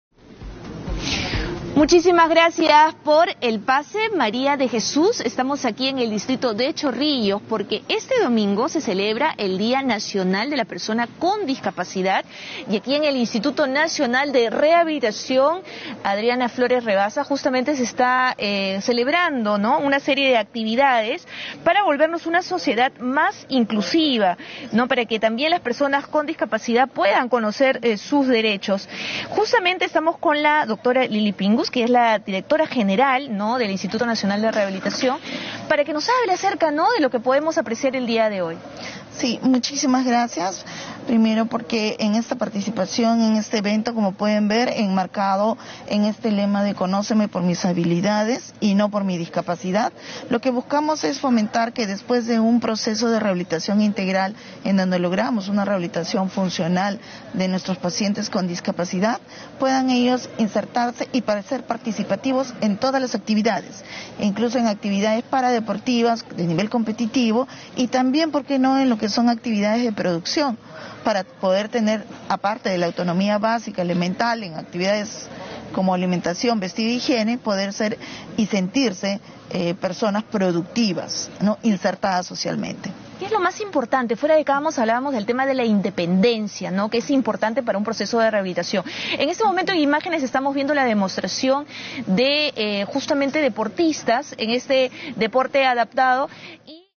Exhibición de disciplinas paradeportivas del INR por el Día de la Persona con Discapacidad
Exhibición de disciplinas paradeportivas del Instituto Nacional de Rehabilitación "Dra. Adriana Rebaza" AMISTAD PERÚ-JAPÓN, en conmemoración por el Día de la Persona con Discapacidad

Fuente: TVPerú